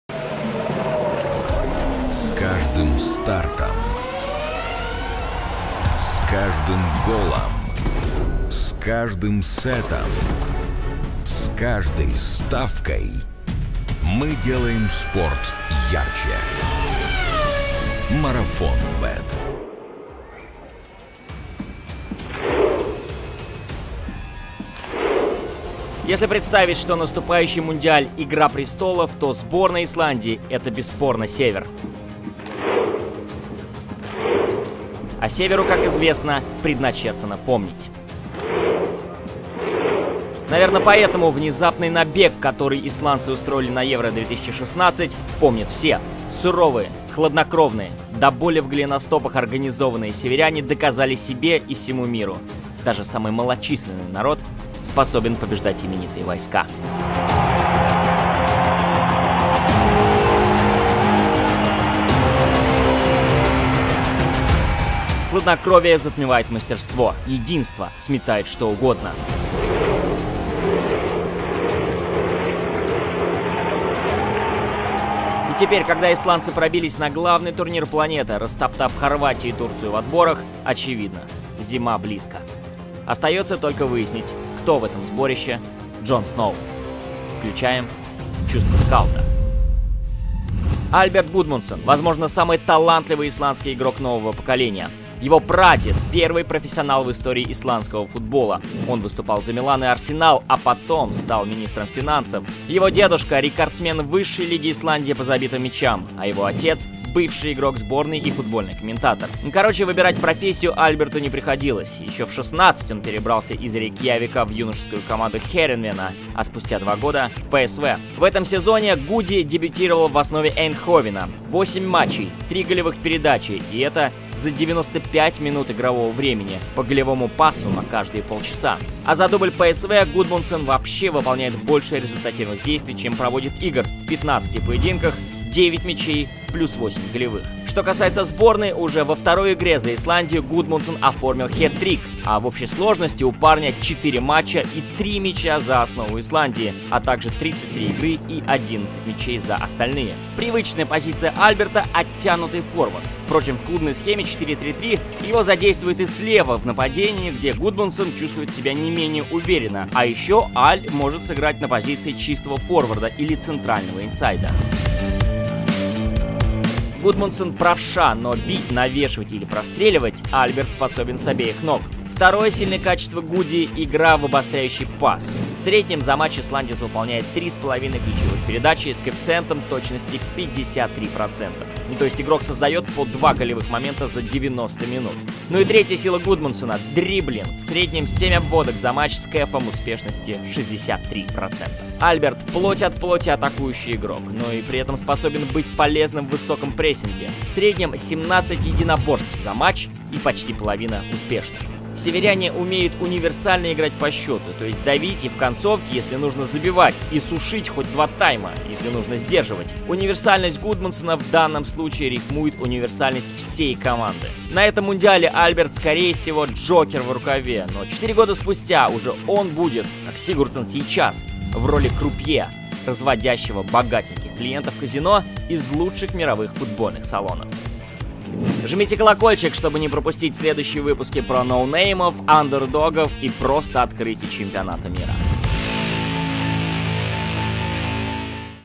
Cборная Исландии на ЧМ | Новый Сигурдссон и ЧУВСТВО СКАУТА
Кто продолжит исландское чудо?!
Новый проект ОСПОРТЕ.ТВ о потенциальных открытиях Чемпионата мира. Ноунеймы и андердоги в составе всех участников: ищем будущих звезд мирового футбола в заявке каждой сборной!